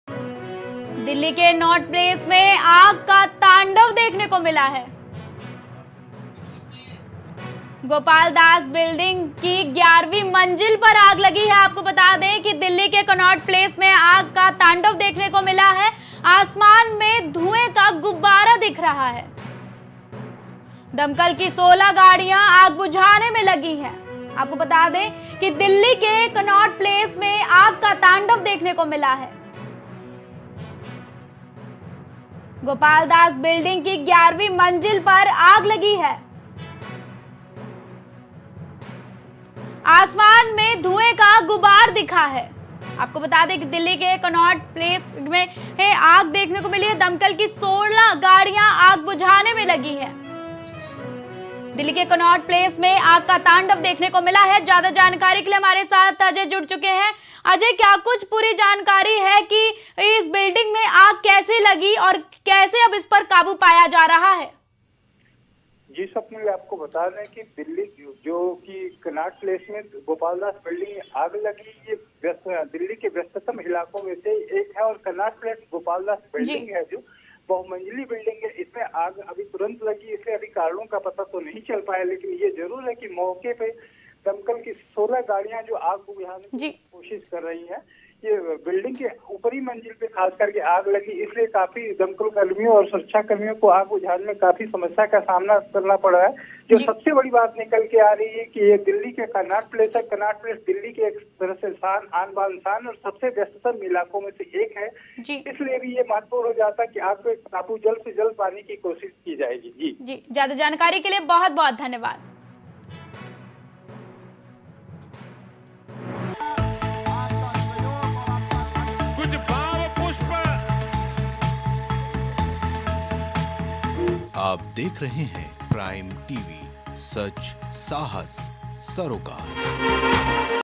Breaking News: Delhi के कनॉट प्लेस में आग का तांडव, 11वीं मंजिल पर आग लगी
Breaking News: Fire broke out in Delhi's Connaught Place, fire broke out on the 11th floor